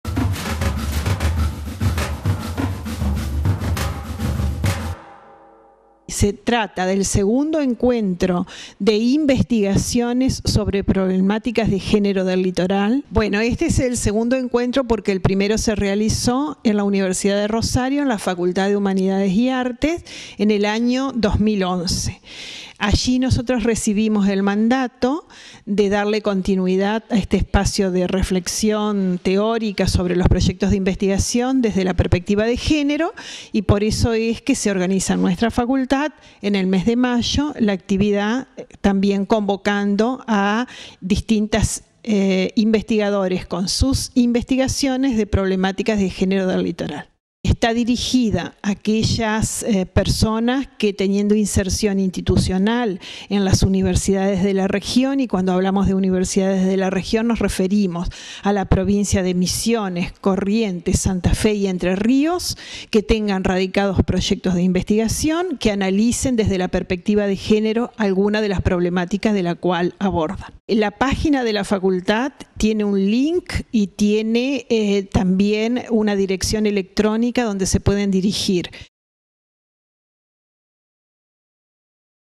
2º Encuentro - Problemáticas de Género del Litoral
El 2º Encuentro de Investigadores sobre problemáticas de Género del Litoral, será el 23 y 24 de mayo, en la Facultad de Trabajo Social. Entrevista con la Mg. Nora Das Biaggio, integrante del Comité Académico.

El Encuentro tiene por objetivo generar un espacio de convergencia entre las/los investigadoras/es de la región que permita el intercambio y debate de las diversas producciones académicas, además de conocer el estado de debate de las investigaciones de género en la región.

Podrán participar proyectos de investigación, en curso o finalizados, que aborden la problemática de las mujeres y del género desde diferentes perspectivas disciplinares.

ARANCELES:
EXPOSITOR GRADUADO: $ 120
EXPOSITOR ESTUDIANTE: $ 80
ASISTENTE GRADUADO: $ 60
ASISTENTE ALUMNO: $ 40

Fuente: Área de Comunicación Institucional - Facultad de Trabajo Social
Realización entrevista: Área de Comunicación Institucional - Facultad de Trabajo Social